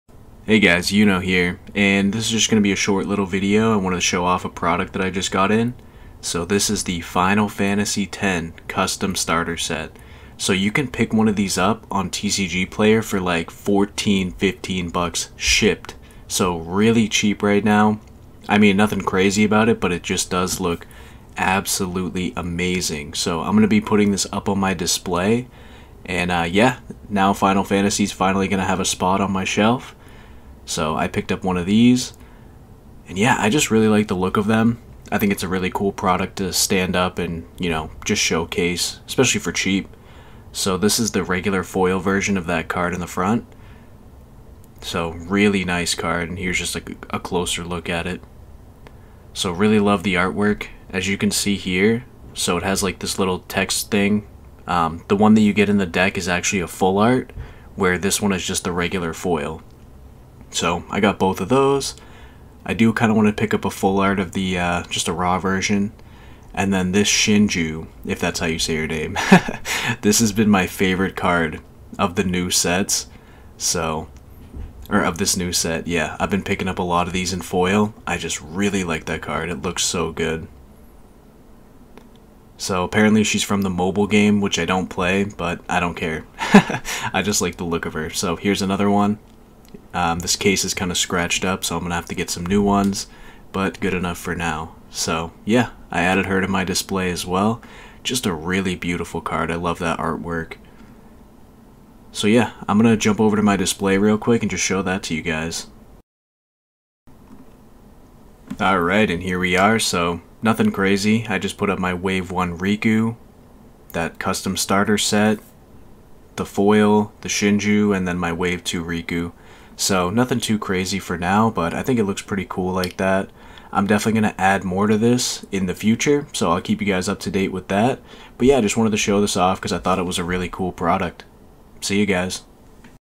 FFTCG FFX Starter Set + Small Pickups Final Fantasy TCG Collection Update
If you are interested in whats inside these starter sets check out this video:

Just wanted to share this really cool product with you guys, I think it looks amazing as a display piece for the price and there are plenty available if anyone out there wants to score one